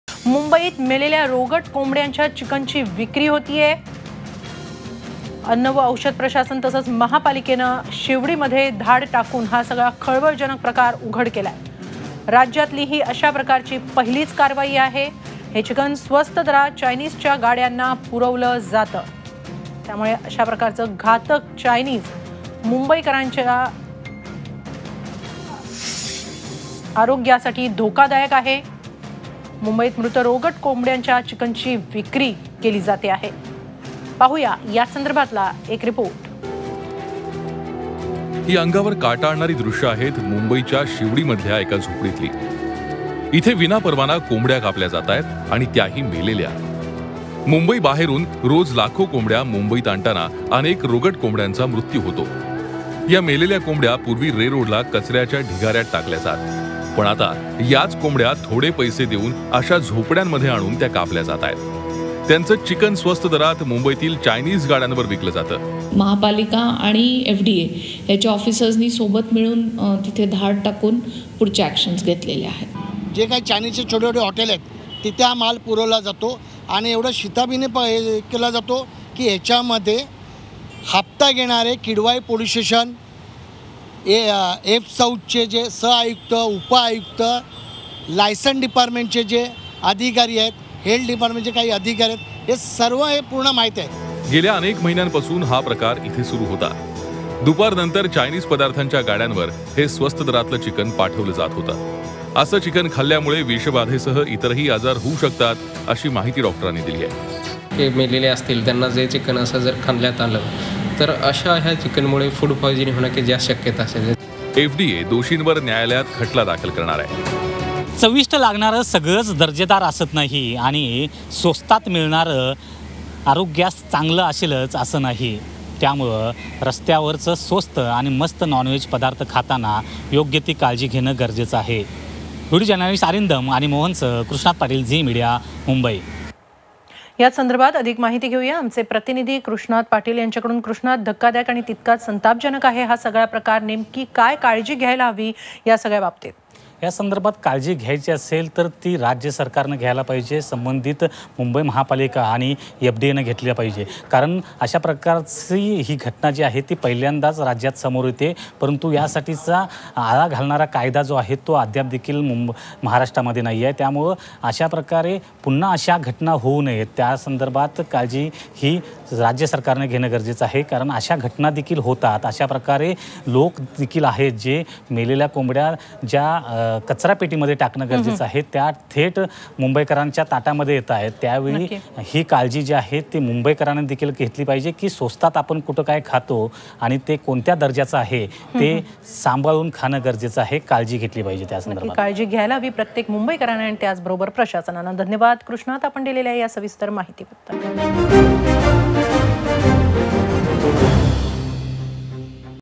सावधान, चायनीज फूडसाठी वापरतायत, मृत, रोगट चिकन
Mumbai | People Be Cautious And Alert To Risk Of Eating Roadside Nonveg Chicken Chinese Food

मुंबई : एक धक्कादायक बातमी नॉन व्हेज खाणा-यांसाठी .मुंबईत मेलेल्या रोगट कोंबड्यांच्या चिकनची विक्री होतेय... एफडीए आणि महापालिकेनं शिवडीत धाड टाकून हा खळबळजनक प्रकार उघड केलाय. राज्यातली अशा प्रकारची ही पहिलीच कारवाई आहे. हे चिकन स्वस्त दरात चायनीजच्या गाड्यांना पुरवलं जायचं...